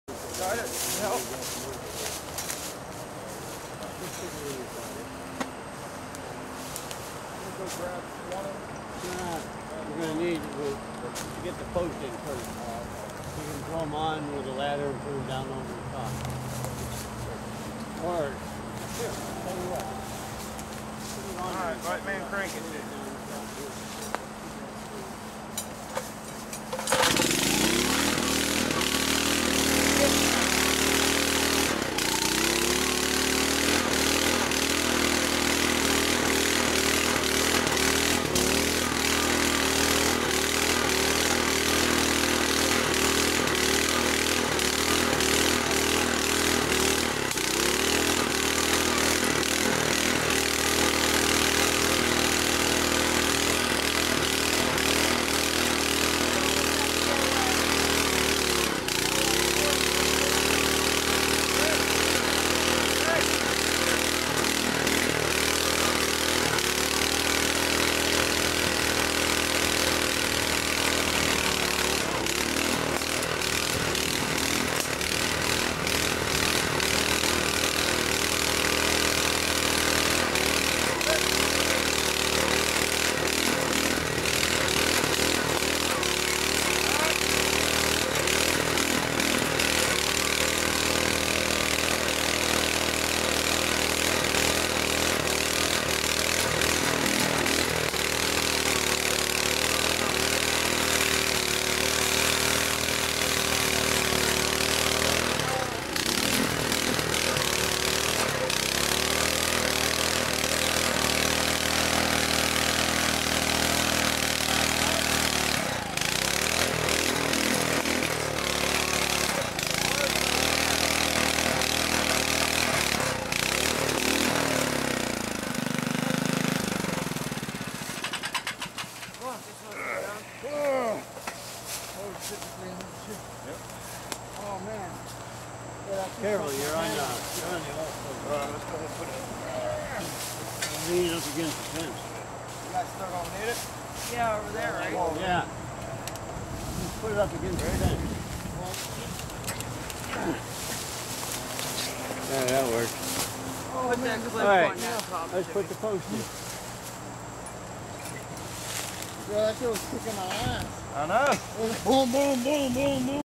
Auger 1
Digging the 4 ft. hole for the fence post with a 2-man auger.